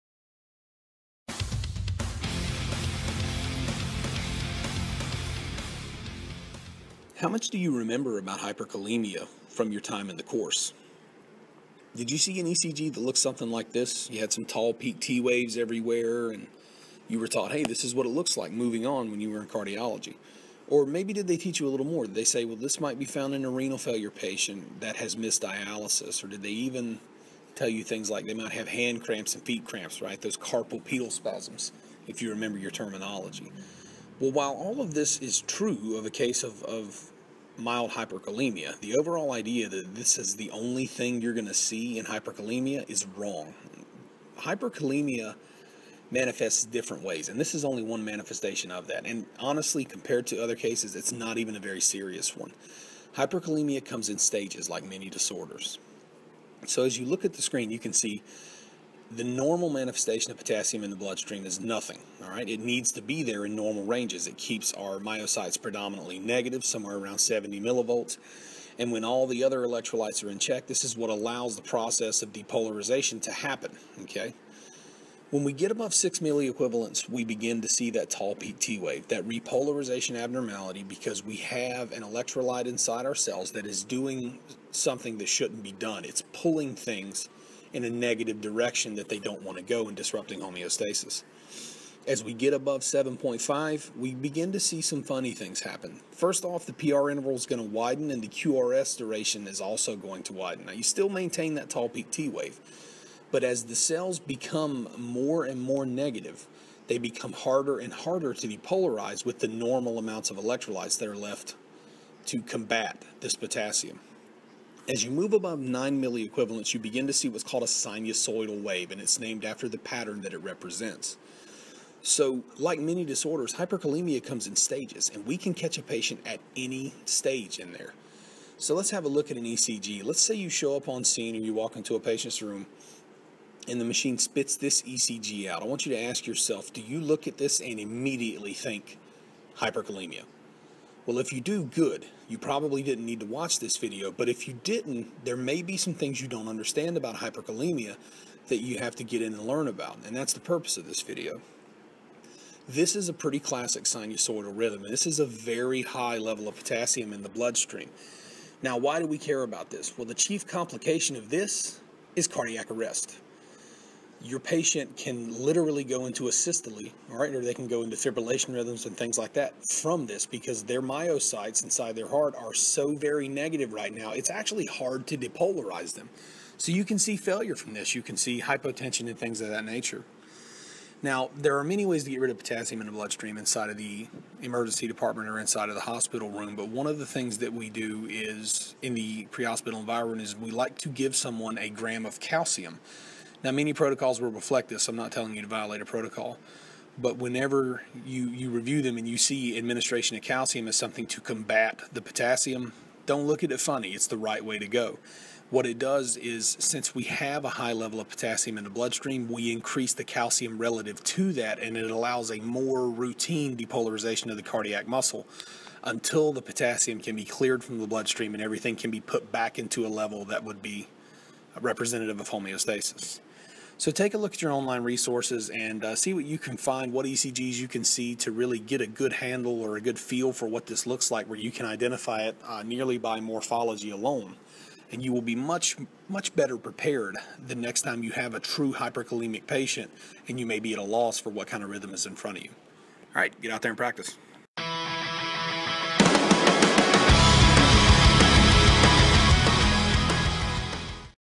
Hyperkalemia: More than peaked T waves
There are several manifestations of hyperkalemia. Sadly, peaked T waves isn't one that we see very often in pre-hospital medicine. Often enough, paramedics look at hyperkalemia ECG without even knowing it because they don't know what the most dangerous stages of hyperkalemia look like.